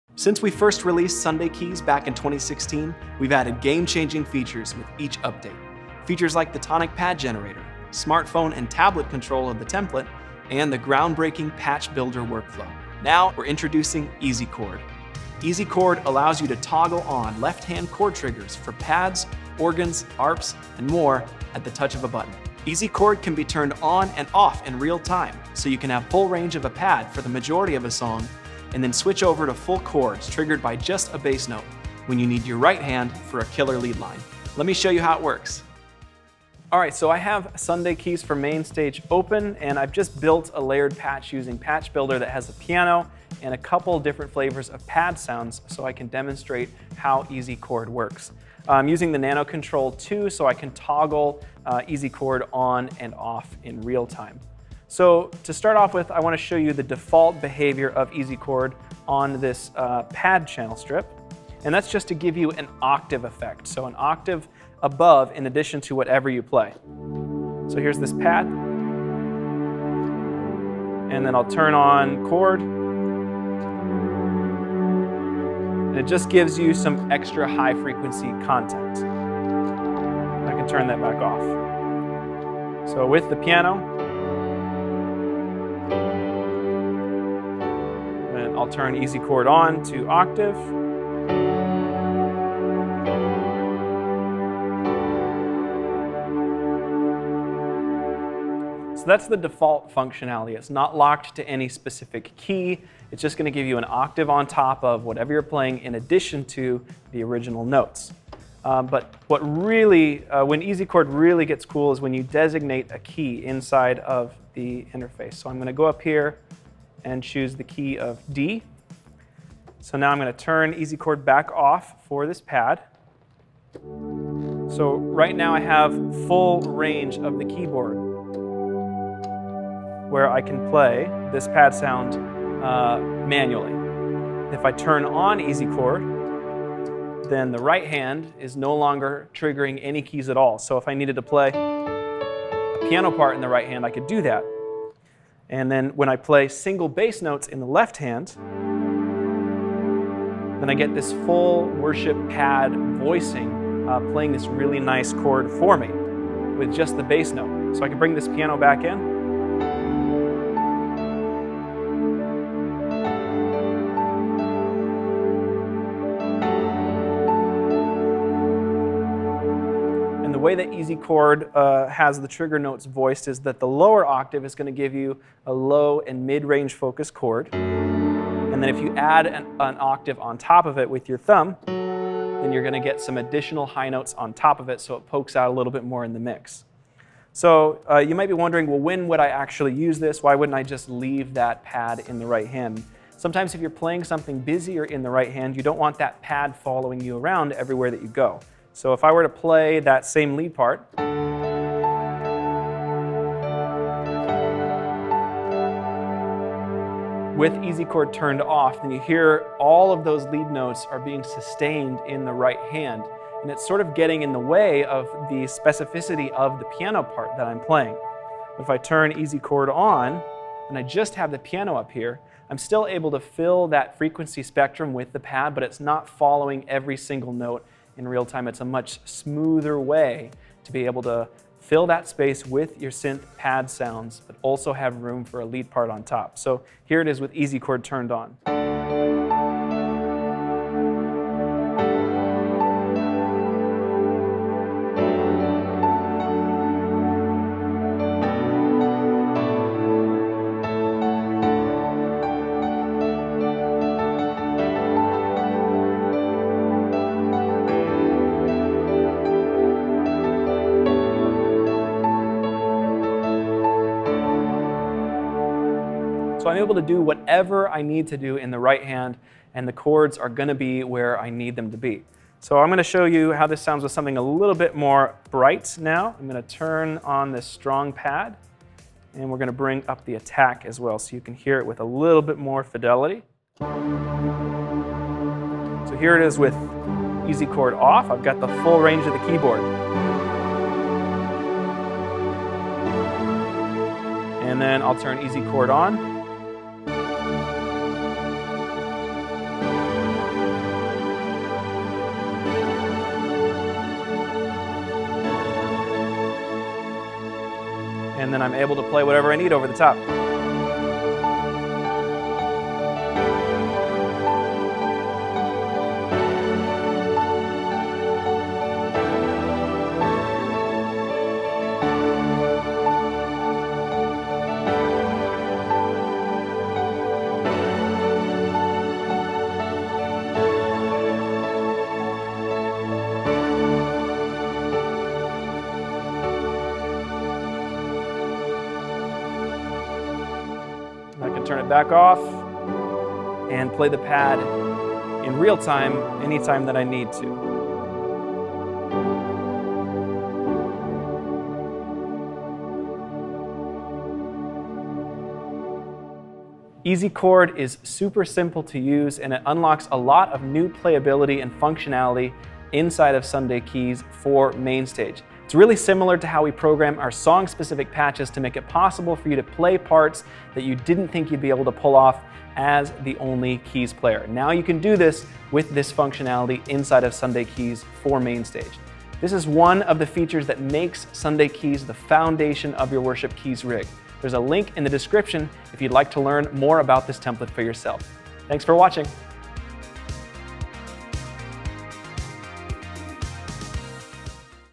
New Easy Chord Feature inside Sunday Keys for MainStage Template
One of the most innovative features we've ever come up with, Easy Chord lets you toggle on full, worship focused chord voicings triggered by your bass notes at the touch of a button.

Easy Chord is without a doubt one of our favorite things we've ever come up with and we know you're gonna love what we've added to the sound library.

The 2020 Edition is a free upgrade for all Sunday Keys owners. Head over to the Sunday Keys Facebook group to download or check your email for all the details.